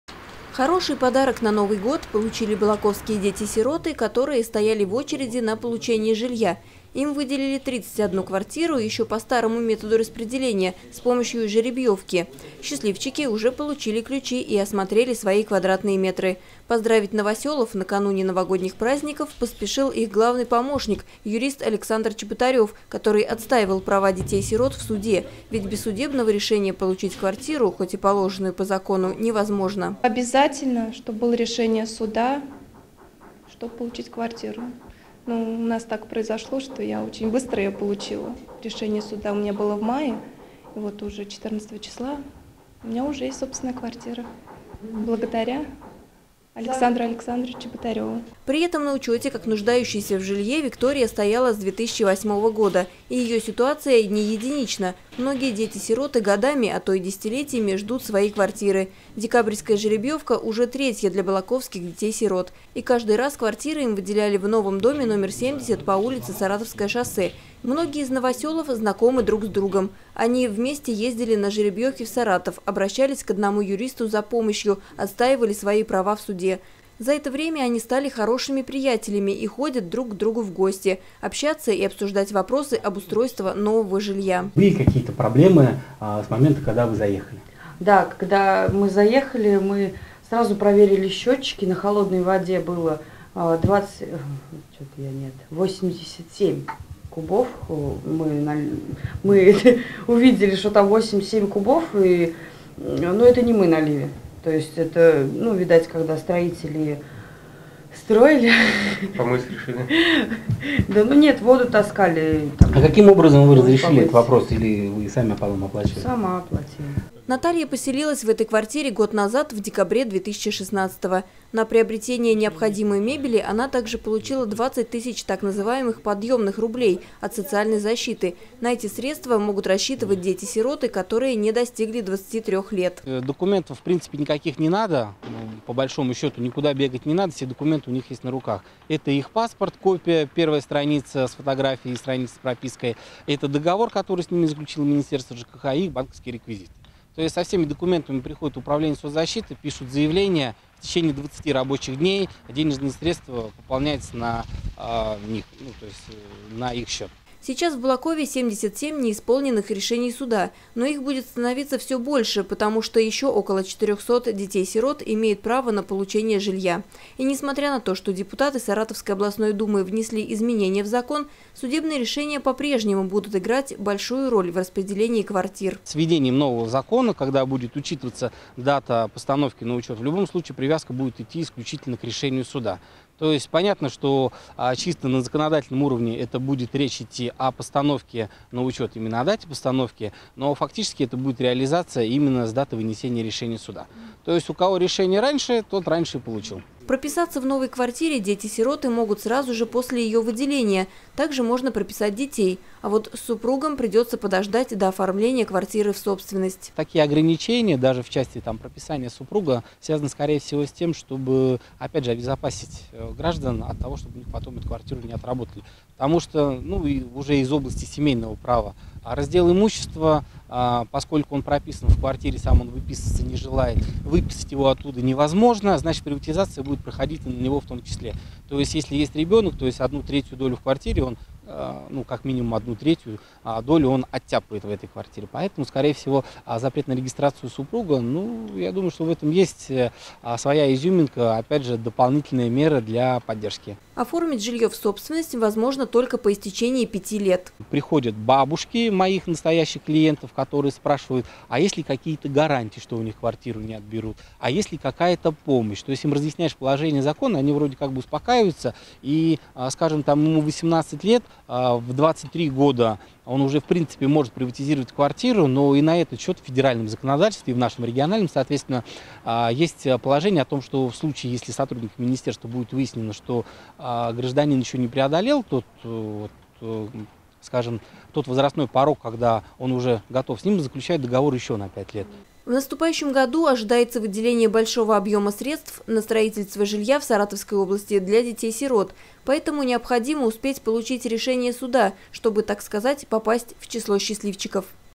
Квартира - через суд. Как будут распределять жилье детям-сиротам в 2018 году?
Хороший подарок на Новый год получили балаковские дети-сироты, которые стояли в очереди на получение жилья – им выделили 31 квартиру – еще по старому методу распределения, с помощью жеребьевки. Однако, на днях Саратовская областная дума внесла изменения в закон, отменив тем самым жеребьевку. Теперь выдача квартир будет проходить по дате постановки на учет. Однако, сиротам по-прежнему придется судиться за свои квартиры - процесс долгий и хлопотный. И тут на помощь приходят юристы. Балаковский юрист Александр Чеботарев, который помогает детям-стротам отстаивать свои права на жилье, дал разъяснения по новому закону.